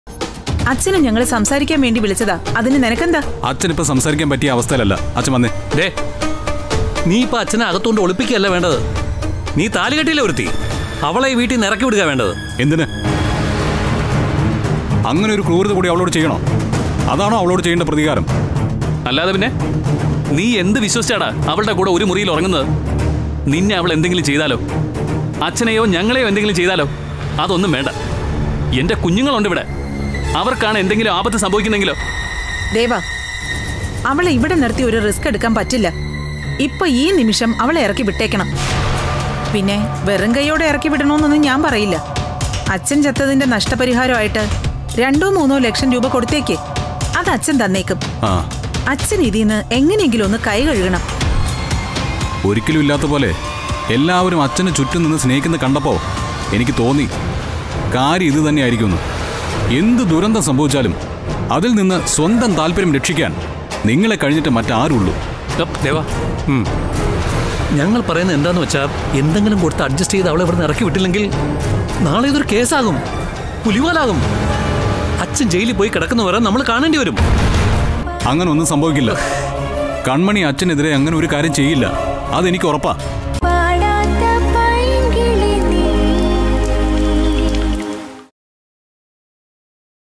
കൺമണിയെ പടിക്ക് പുറത്താക്കാൻ കാരണം കണ്ടെത്തി ഇവർ..
കൺമണിയെ പടിക്ക് പുറത്താക്കാൻ കാരണം കണ്ടെത്തി ഇവർ.. അച്ഛനെ ചേർത്ത് പിടിച്ച് ദേവ